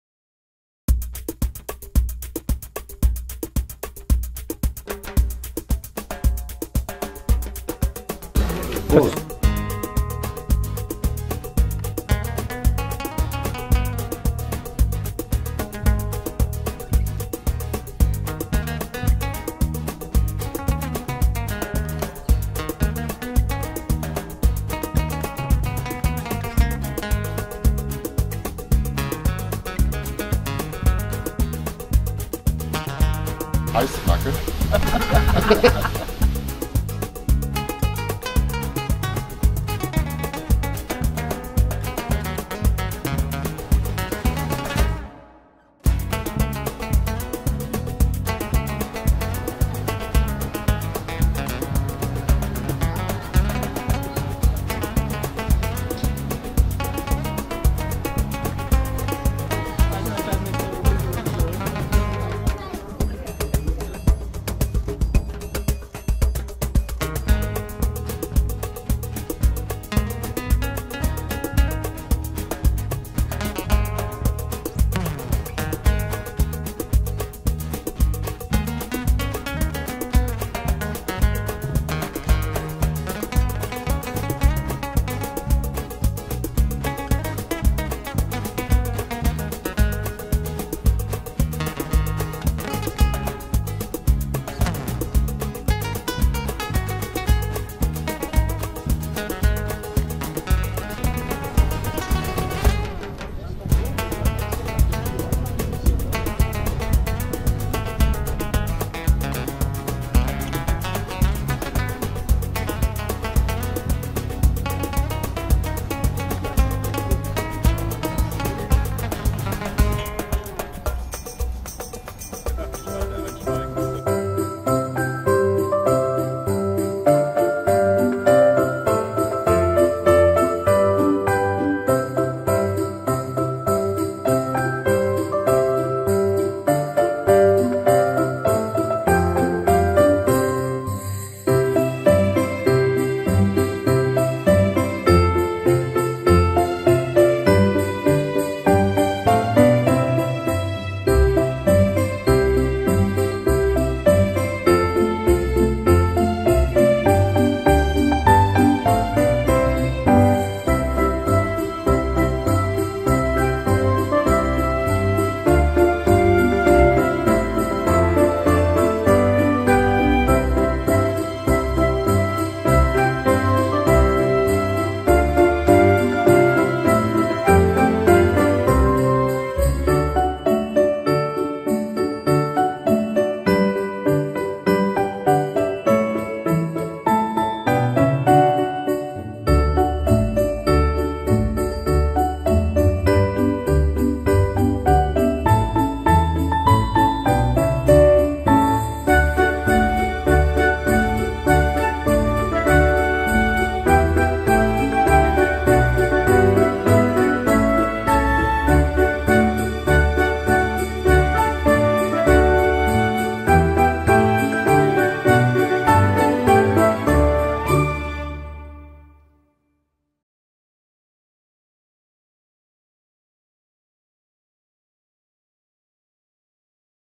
Rhein-Boot Netz Treffen 2009
Die Skipper vom Rhein-Boot Netz treffen sich 2009 an vielen Orten im Südwesten.